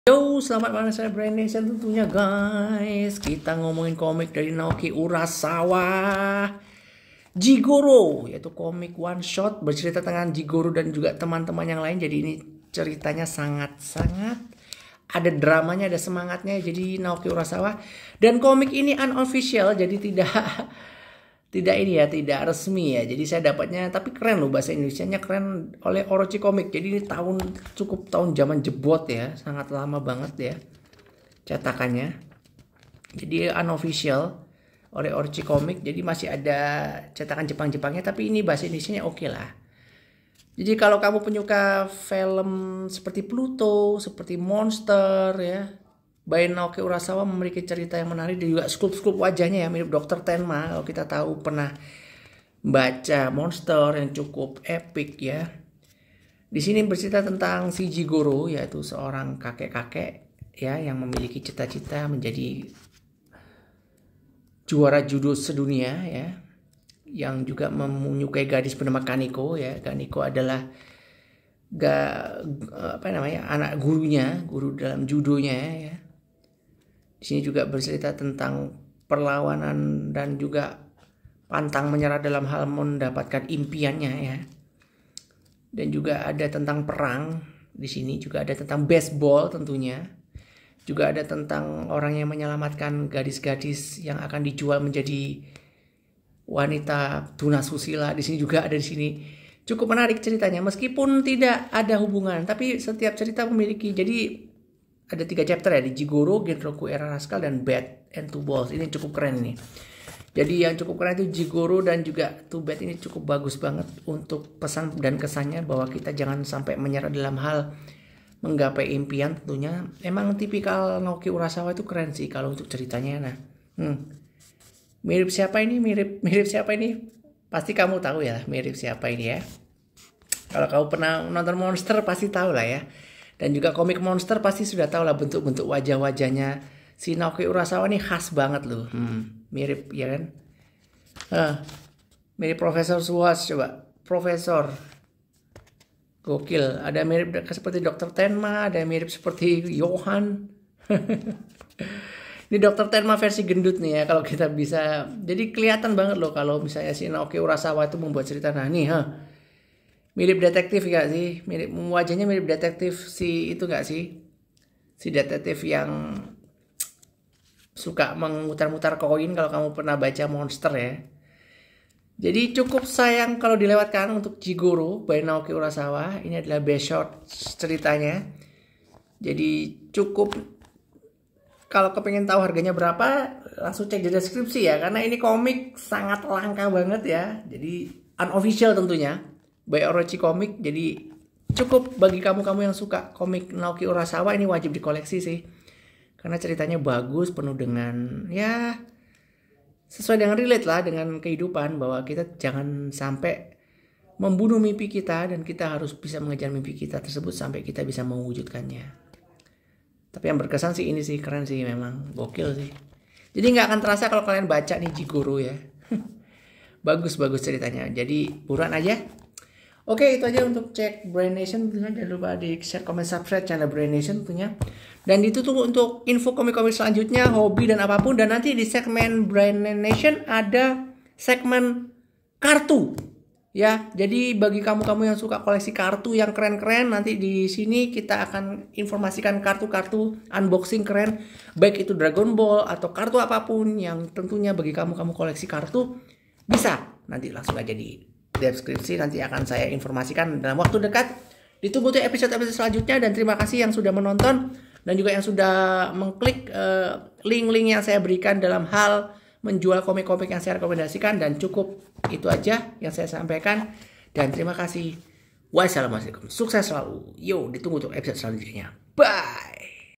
komik jigoro oneshoot dari mangaka naoki urasawa saiko menjadi humor kocak
Untuk pembelian komik yang saya review bisa di beli disini

Cerita lucu dari mangaka naoki urasawa masterpiece tentang pencapaian impian fan kerja keras inspiratif